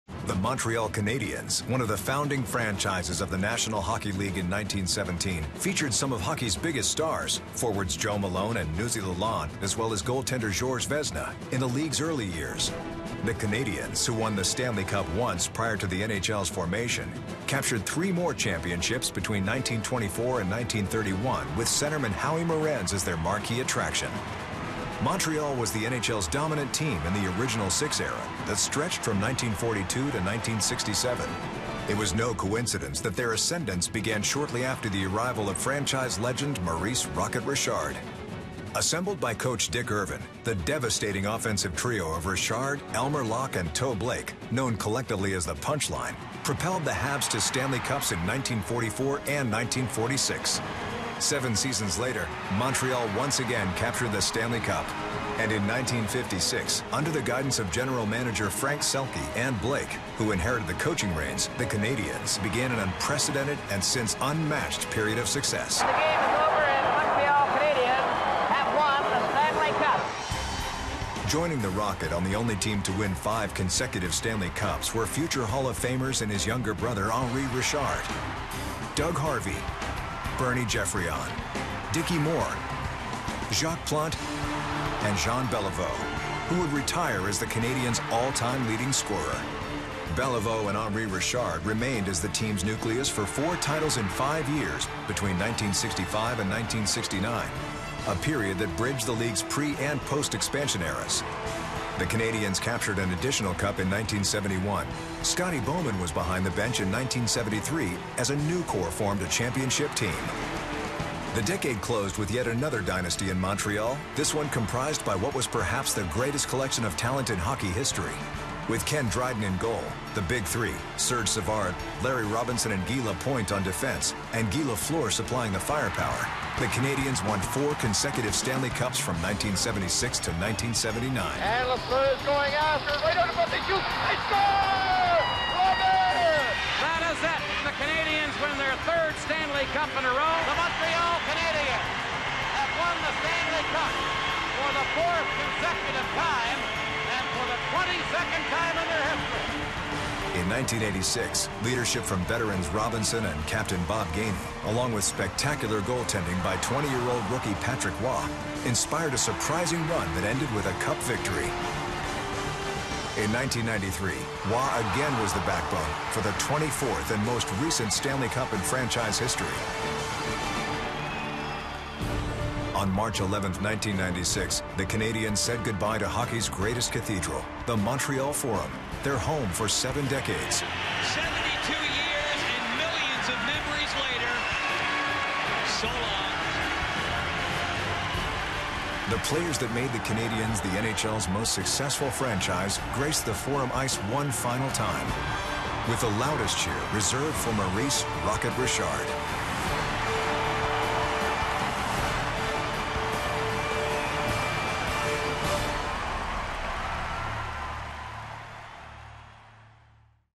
NHL Team Histories: Montreal Canadiens
Winners of 24 Stanley Cups, and with names like Beliveau, Richard and Roy, the Montreal Canadiens are perhaps the NHL's most storied franchise.

Presented by NHL.com and Tim Horton.